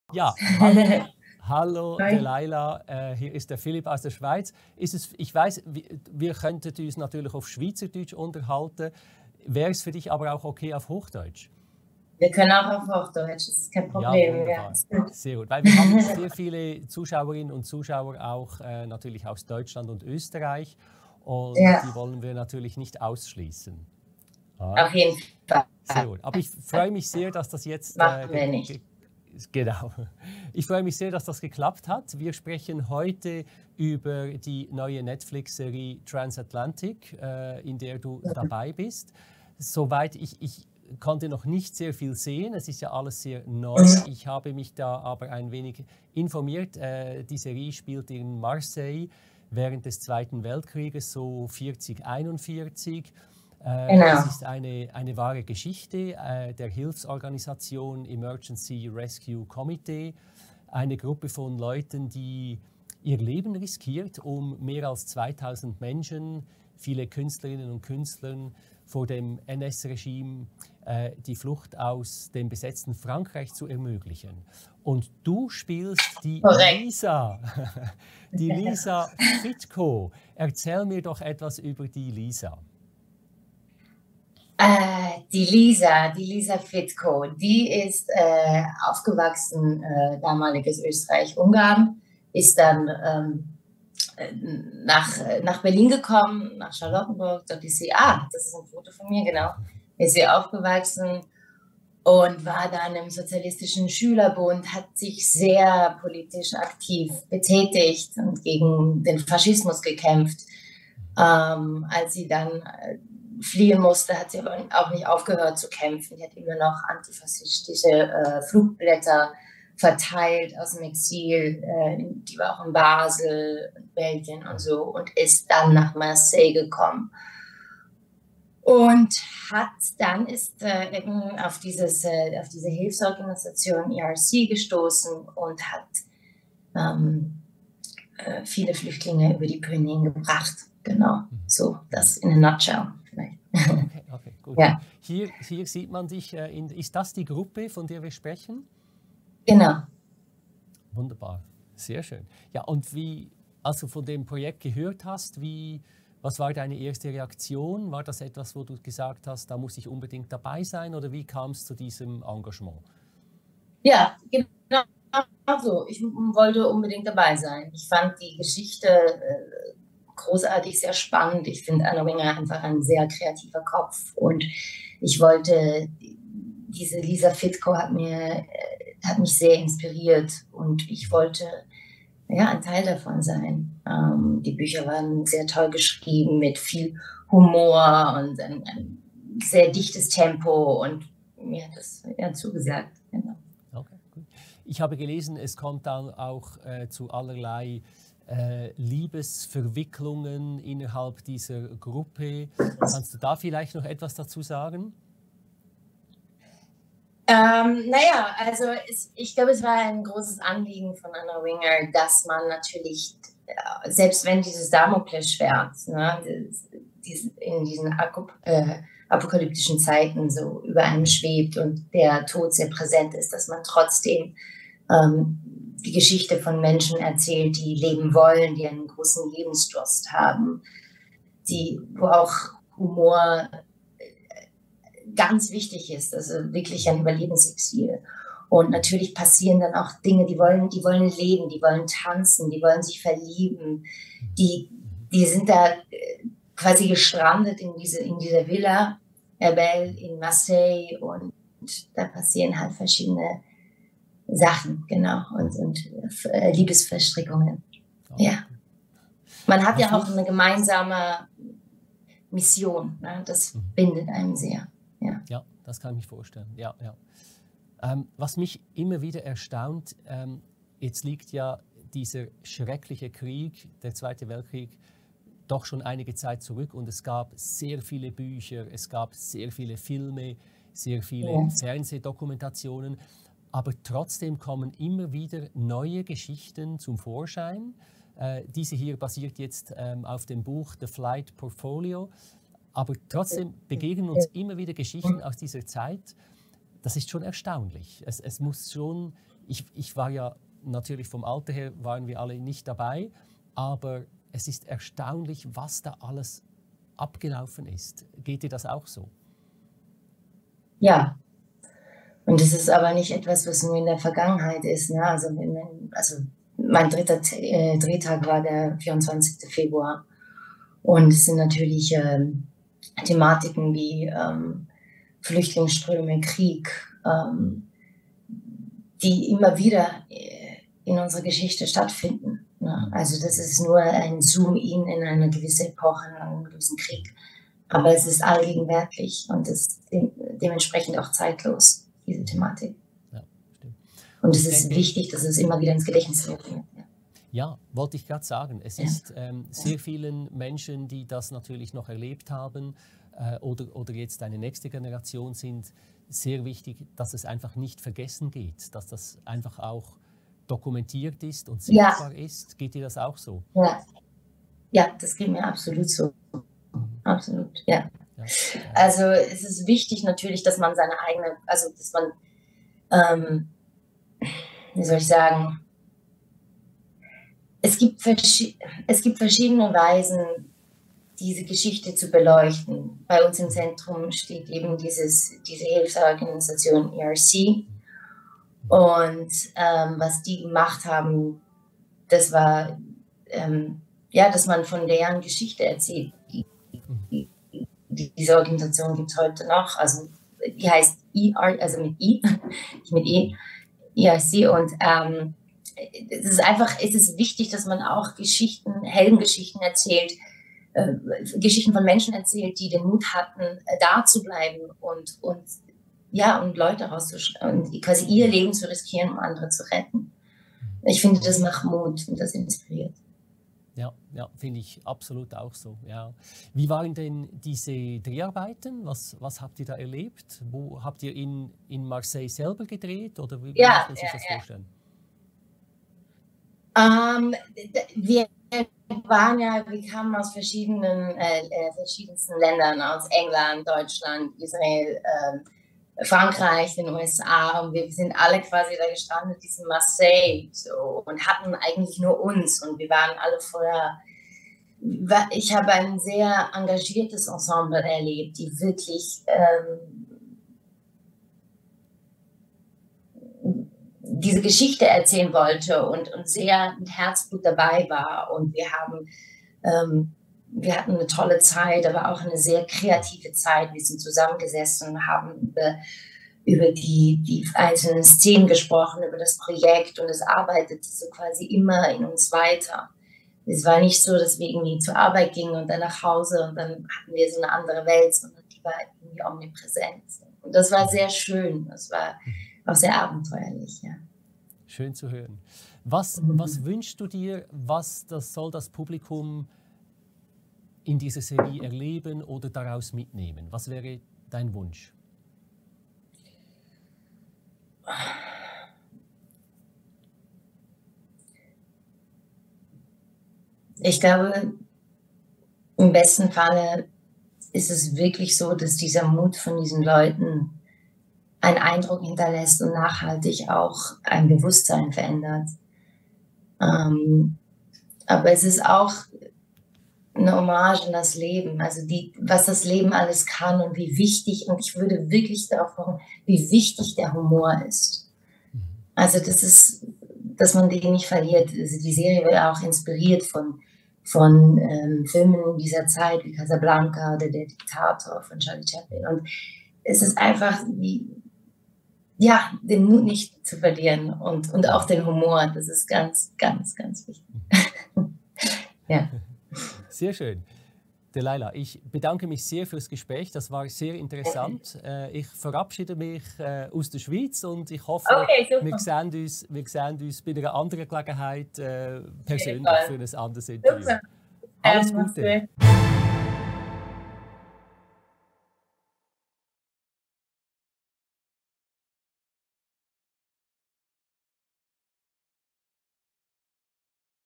Deleila Piasko TRANSATLANTIC Interview *Netflix*
Hallo! Viel Spass beim Schauen :)

0:00 Intro
1:24 Über ihre Figur Lisa
2:43 Ihre Reaktion, als sie vom Projekt erfuhr
3:26 Die Liebesverwicklungen in der Serie
5:10 Die Aktualität der Thematik
6:06 Heldengeschichten
7:15 Über die Dreharbeiten
8:26 Die Botschaft der Serie

Und hier für alle Filmprognosen zu den neusten Filmen und Serien